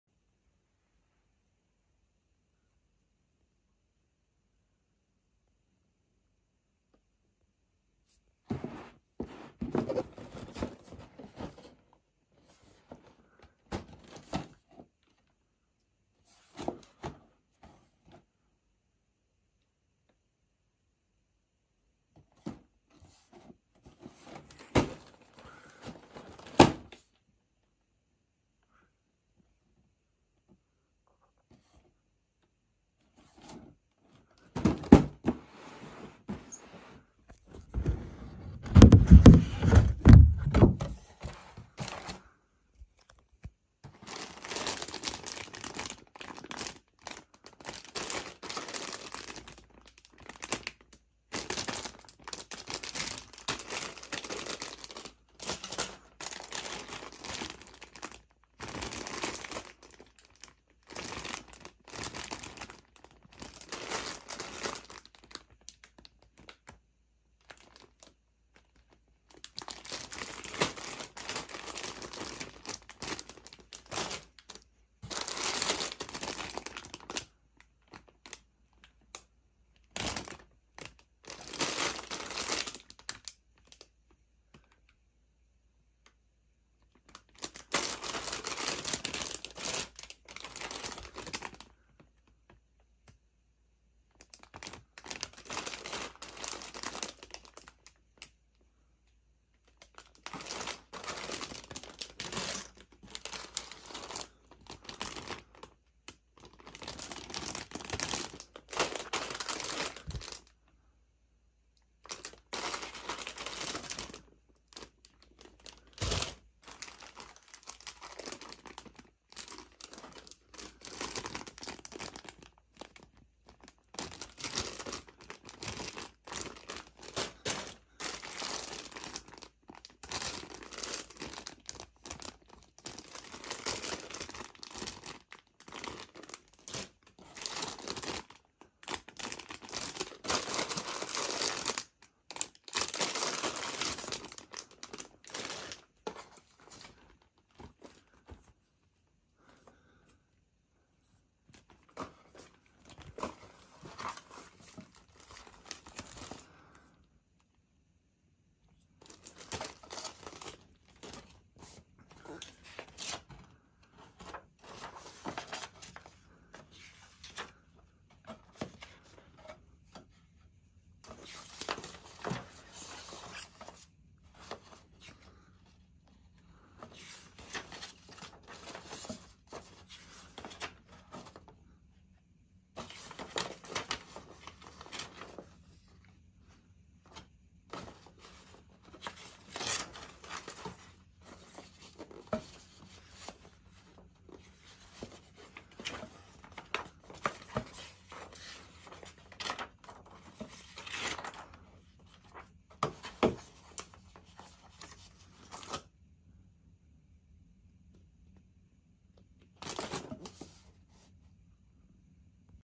Alphamon figure rise standards amplified unboxing digimon
He is one of my favorite digimon the box looks huge it’s very interesting.